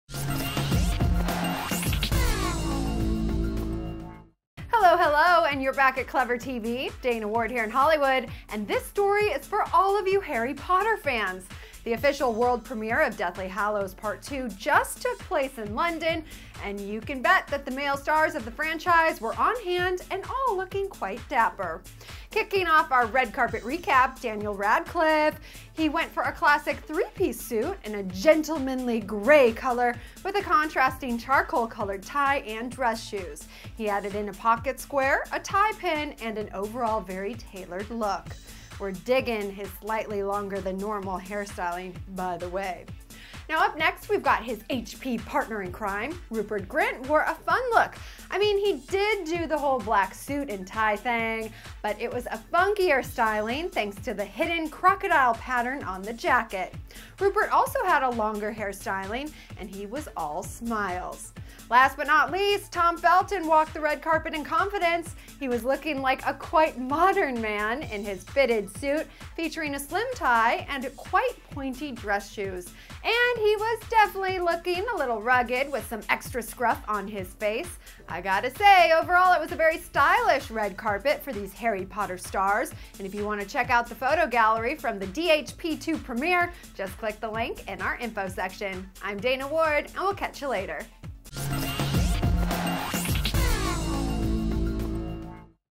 'Harry Potter' Premiere Guys Fashion Recap
Hello hello and you're back at ClevverTV. Dana Ward here in Hollywood and this story is for all of you Harry Potter fans. The official world premiere of Deathly Hallows Part 2 just took place in London, and you can bet the male stars of the franchise were on hand and all looking quite dapper. Kicking off our red carpet recap, Daniel Radcliffe. He went for a classic 3-piece suit in a gentlemanly grey color with a contrasting charcoal colored tie and dress shoes. He added in a pocket square, tie pin and an overall very tailored look. We're digging his slightly longer than normal hair styling. Up next, we've got his HP partner-in-crime. Rupert Grint wore a fun look. I mean, he did do the whole black suit and tie thang, but it was a funkier styling thanks to the hidden crocodile pattern on the jacket. Rupert also had a longer hair styling and he was all smiles. Last but not least, Tom Felton walked the red carpet in confidence. He was looking like quite a modern man, in his fitted suit, featuring a slim tie and quite pointy dress shoes. And he was definitely looking a little rugged with some extra scruff on his face. Overall a very stylish red carpet for these Harry Potter stars. If you want to check out the photo gallery from the DHP2 premiere, just click the link in our info section. I'm Dana Ward and we'll catch ya later!